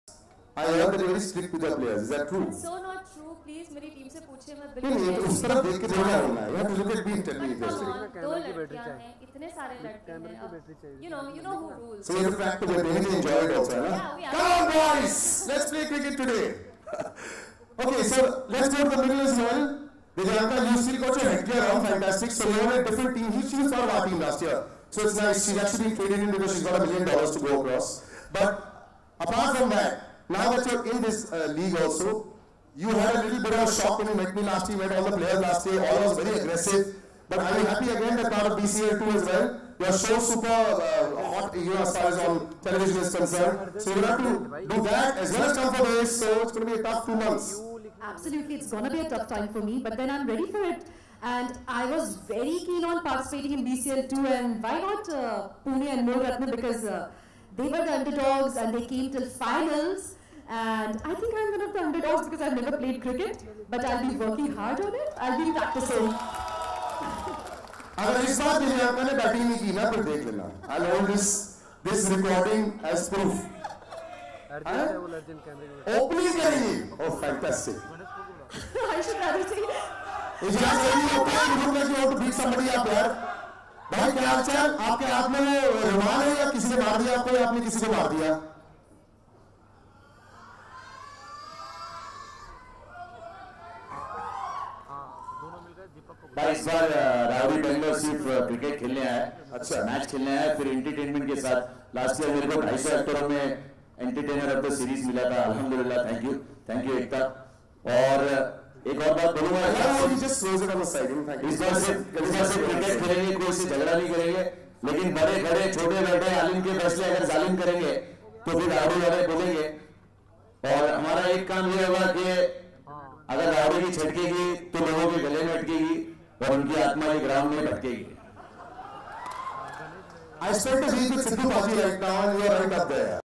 Sunny Leone And Yeh Hai Mohabbatein's Cast In Ekta Kapoor's 'Box Cricket League Season 2'
Watch Participants Of Box Cricket League Season 2 In Interview.